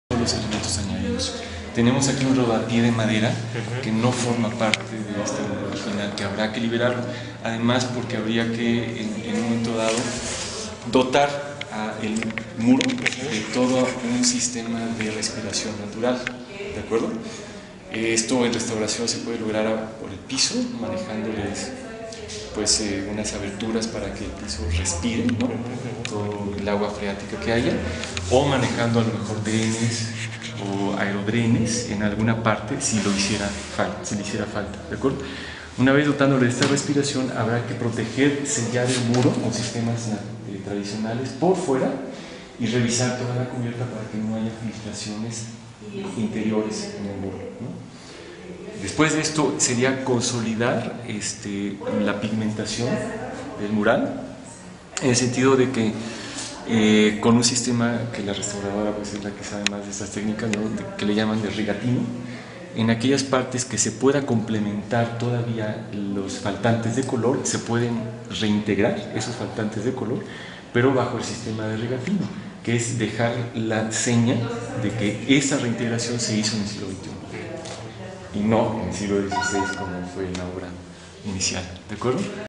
Preparan restauración de recintos coloniales en Edomex
Cinco inmuebles coloniales del Edomex, cuatro menos que en 2011, serán restaurados mediante el Foremoba, con un monto de 3 millones 779 mil 473 pesos.